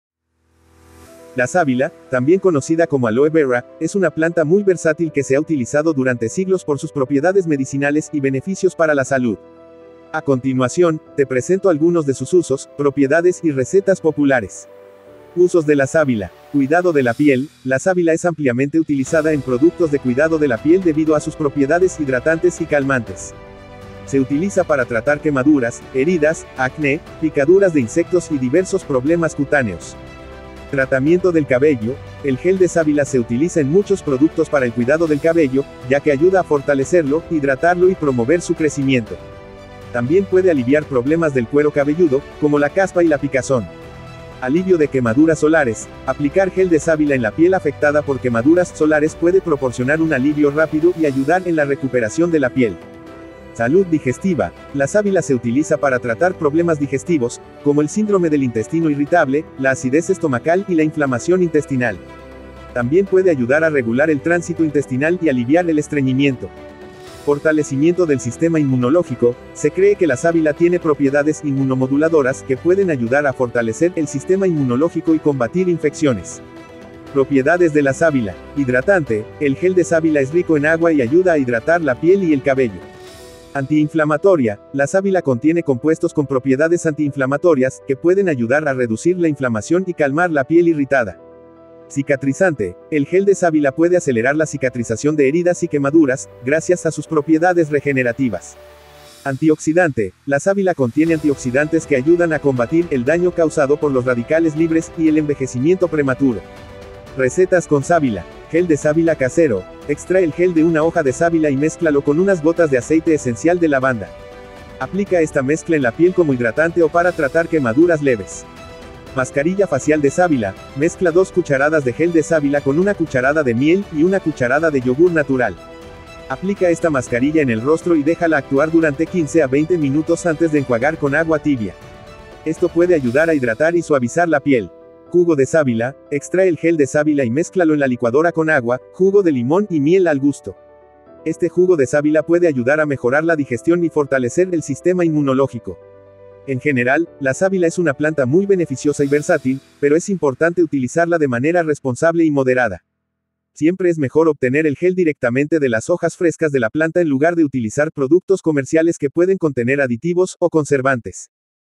sábila (aloe vera) Usos y recetas
En este video les comparto usos, propiedades y rectas del aloe vera,una planta medicinal empleada desde hace miles de años.